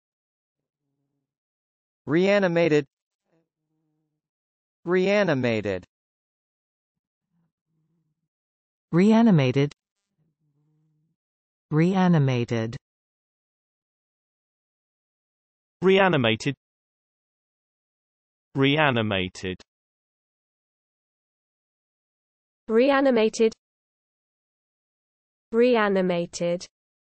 How to pronounce reanimated
Learn how to say Reanimated in English correctly with TextToSpeech.io free pronunciation tutorials. American & British English pronunciation of male & female voices provided in videos. Check them out!

    Reanimated definitions:
    (s): given fresh life or vigor or spirit; "stirred by revived hopes"